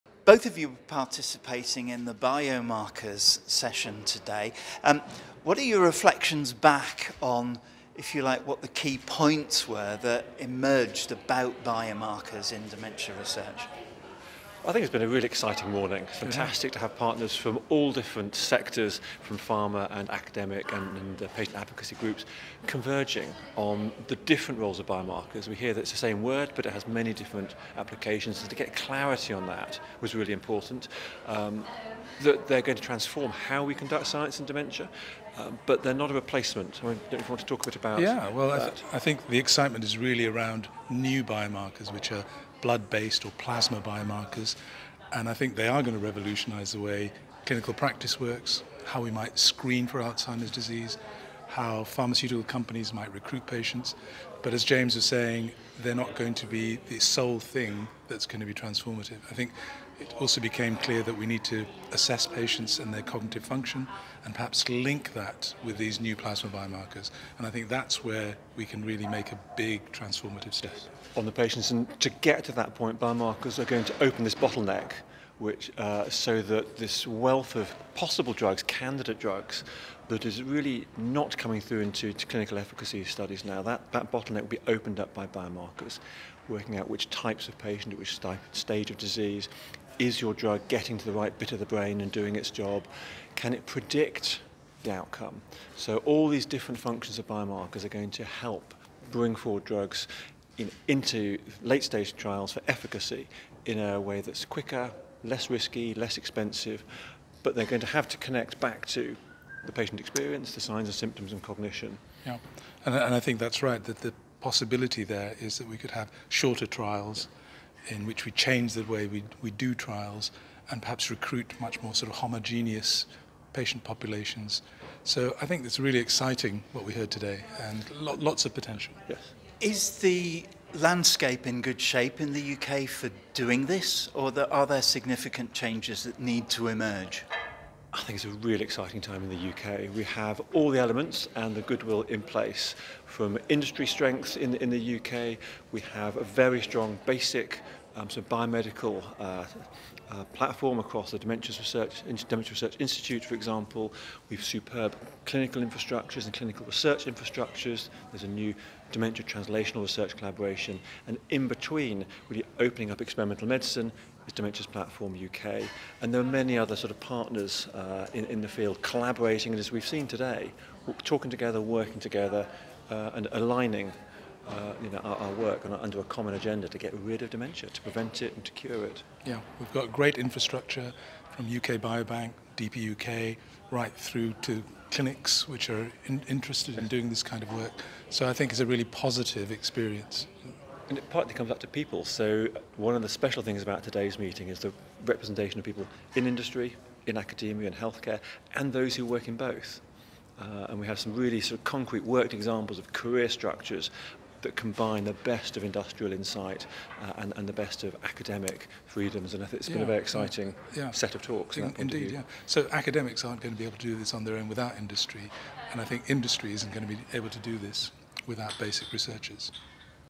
CONFERENCE REFLECTIONS - BIOMARKERS - Prof Masud Husein & Prof James Rowe
Professor Masud Husain (University of Oxford) and Professor James Rowe (Cambridge University) talk about the themes which emerged from the TRANSLATION 2023 session on biomarkers in dementia research.

The TRANSLATION 2023 conference was held over 2 days at the British Medical Association’s headquarters in London. Organised by Dementias Platform UK, it brought together in one place the wide range of researchers working in the field of dementia to explore what the challenges and achievements are and look at the future direction of collaborative working.
The translation of research discoveries, progress through trials, the immense value of big data and the need for good experimental trials are all part of the translation landscape.